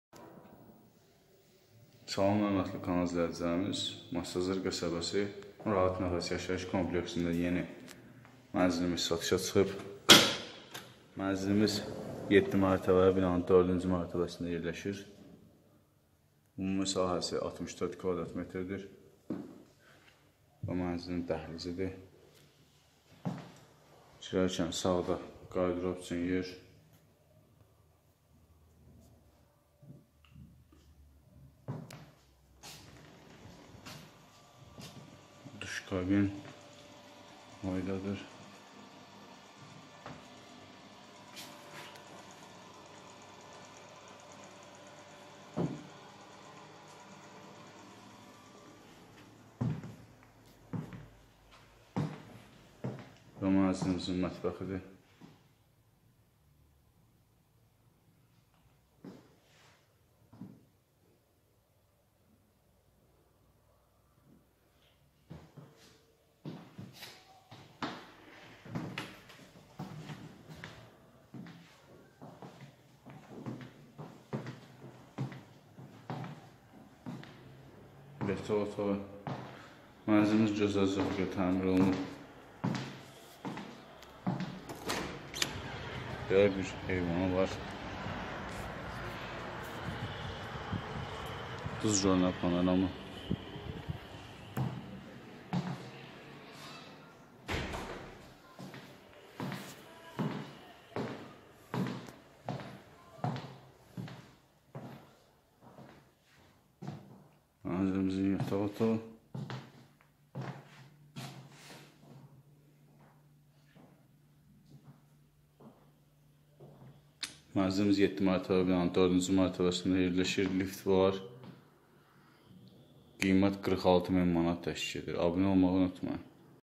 ucuz mənzil satılır.qanuni 2 otaq 64 kv təmirli mənzil.46000azn 055-240-18-41 Zaur
Masazır qəsəbəsi Rahat Nəfəs yaşayış kompleksində 7 mərtəbənin 4 cü mərtəbəsində 2 otaq 64 kv təmirli mənzil satılır.Mətbəx mebeli var.Lift işlək.qiymət: 46000azn                                 ☎️ 050/070/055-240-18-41 Zaur                             Növbəti elanları izləmək üçün kanala abunə olun.